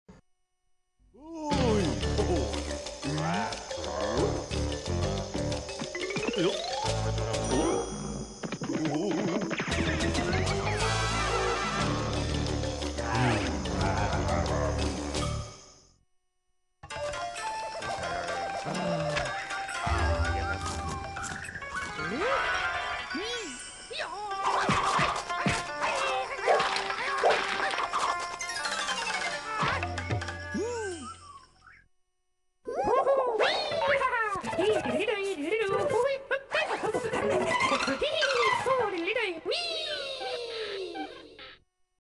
Checkers 2
A series of Idents for Cartoon Network featuring their Checkers logo. Directed by Mohan Subra and Produced by Animata Productions